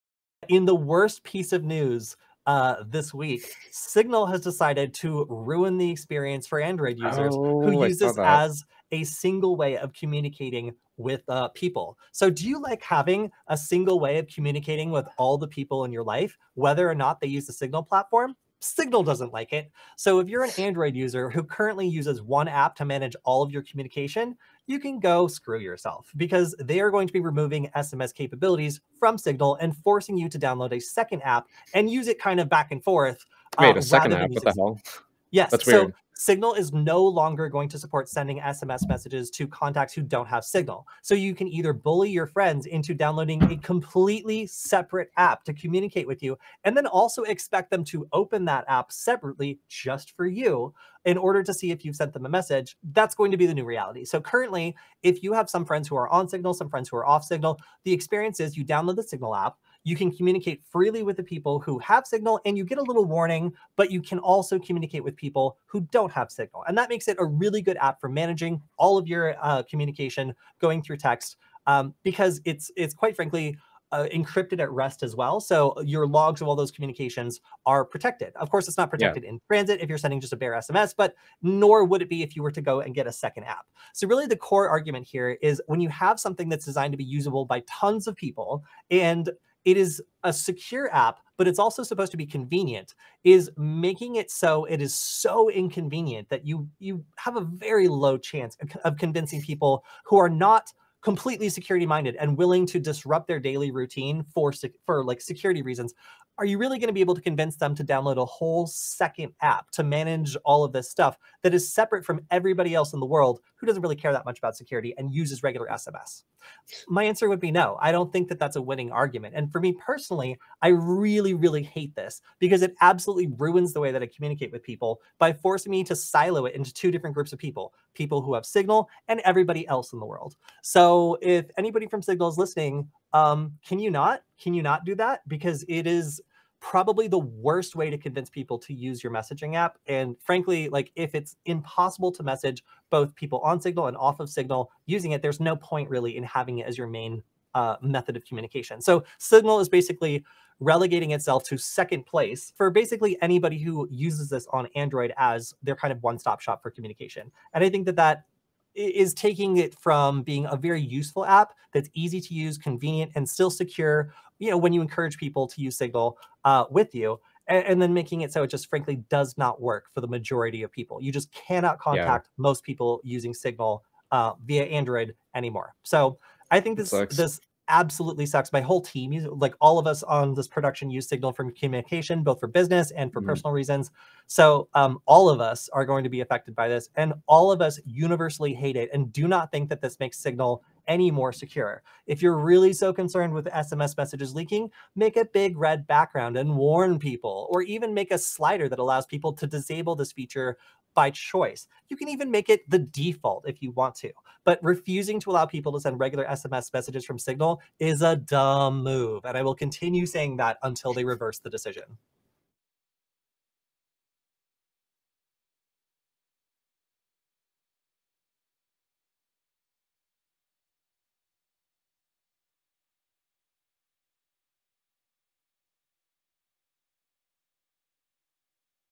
Signal is Shutting out SMS Users
Signal messenger is shutting down SMS support for Android users in a blow to anyone using Signal to manage their communications. While designed to make the service more secure, it also forces users to divide their contacts between Signal and an SMS app.